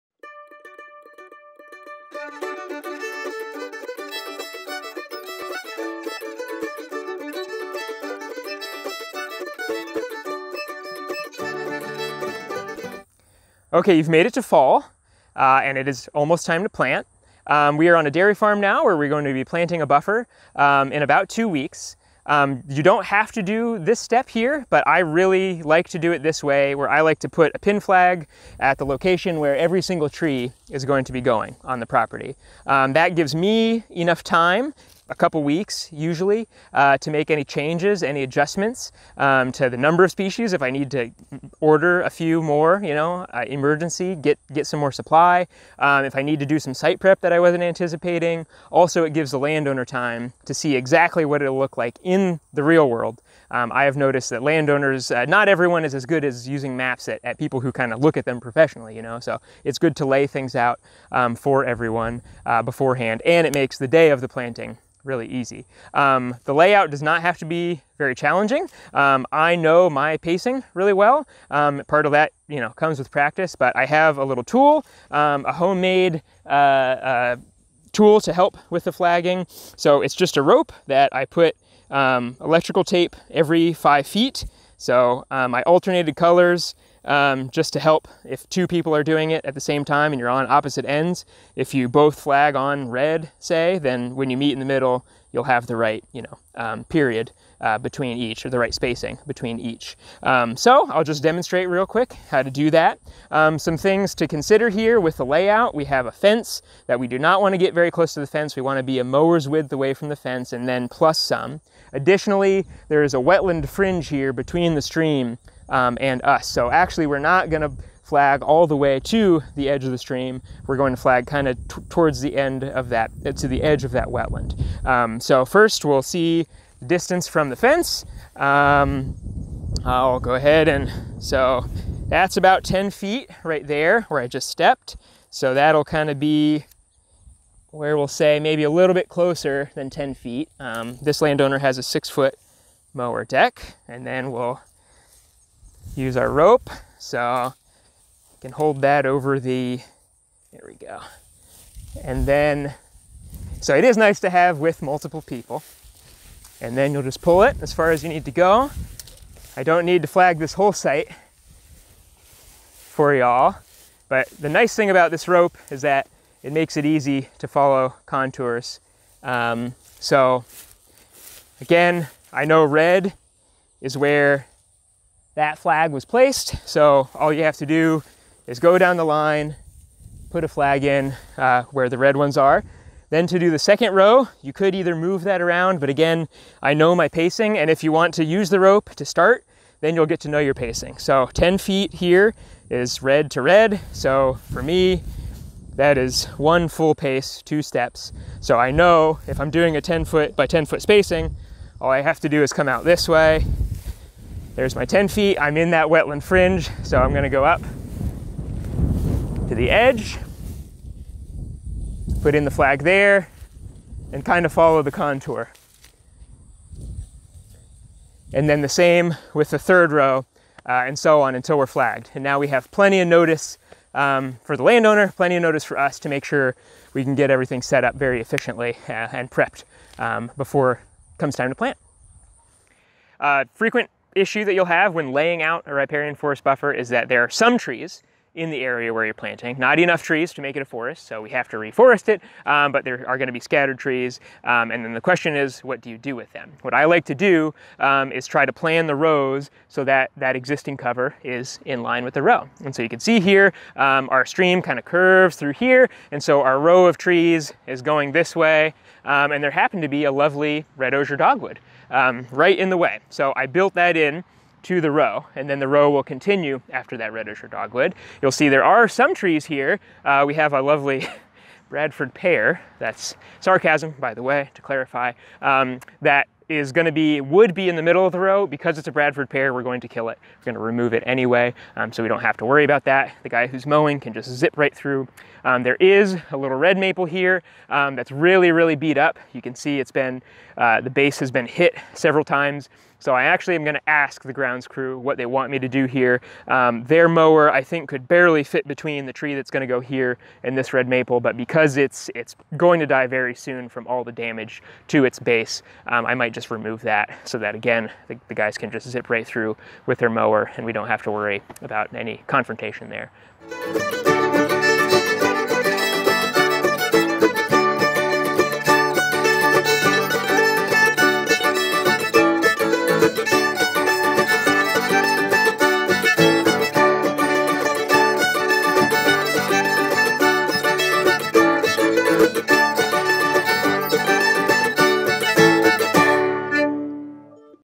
Riparian Restoration 101: Buffer Layout and Flagging
A smart approach to laying out a riparian forest buffer is to flag the location for each tree a couple of weeks prior to planting. This way you guarantee enough space between rows for mowing, give the landowner an opportunity to consider the layout before the trees go in the ground, and serves as a last check for any needed site prep.

In this Riparian Restoration 101, Ryan Davis, Pennsylvania Forest Program Manager, shares his process for flagging a site, and how to accommodate for existing tree cover and site topography.

This series was created by the Alliance for the Chesapeake Bay and the Upper and Middle James Riparian Consortium, and funded by the James River Association.